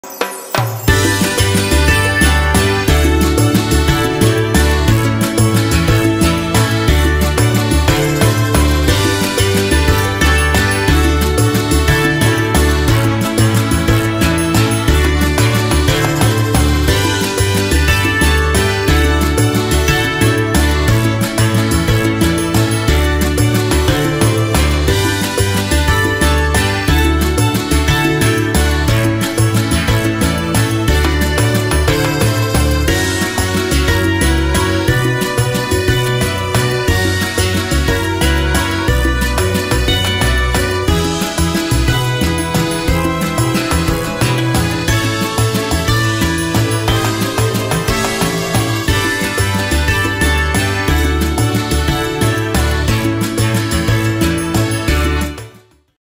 Tuttaunaltrafesta On the Road
La fiera del vivere solidale dei missionari del Pime diventa «on the road» con tanti appuntamenti in giro per la Lombardia. Guarda le immagini della prima data a Inzago (Mi) l'8 aprile 2018 e le date dei prossimi appuntamenti a Varese, Morimondo e Sotto il Monte (Bg)